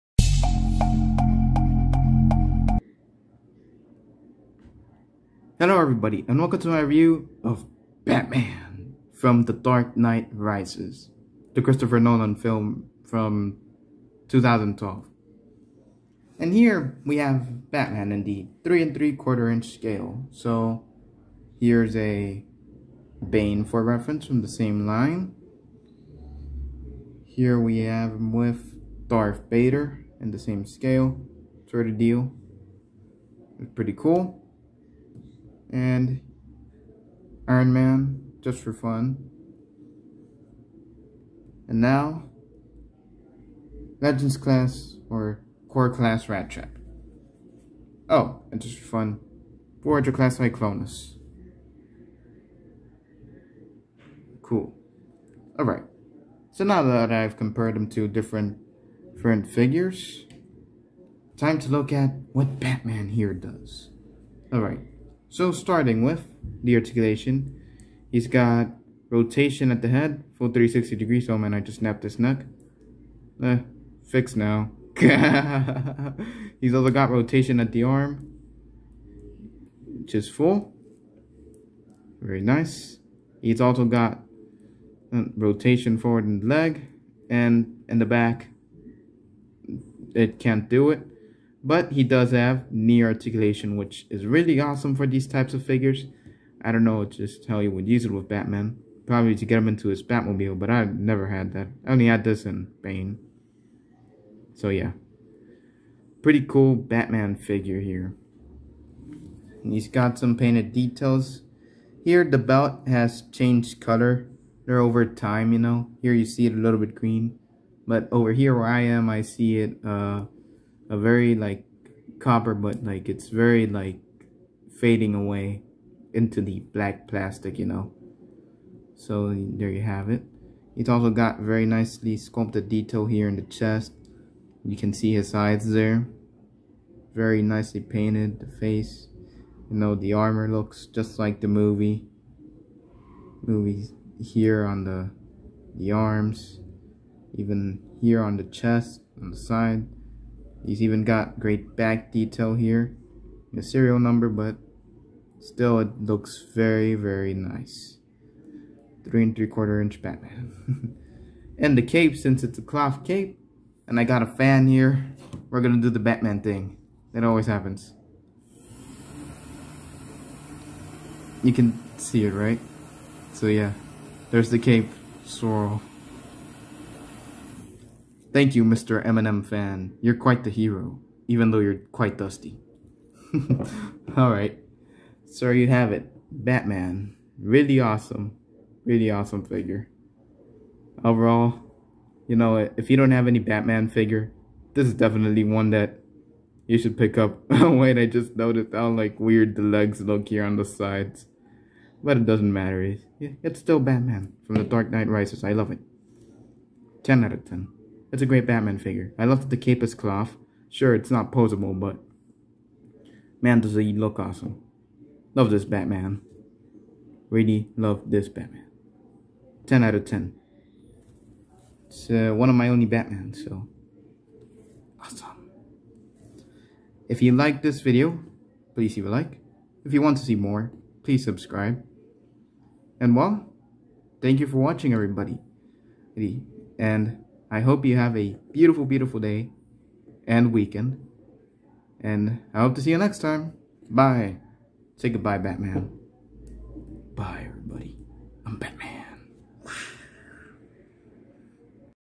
The Dark Knight Rises: Batman Review!
Hello everybody and welcome to my review of one the first Batman figures I got a long time ago! I hope you enjoy!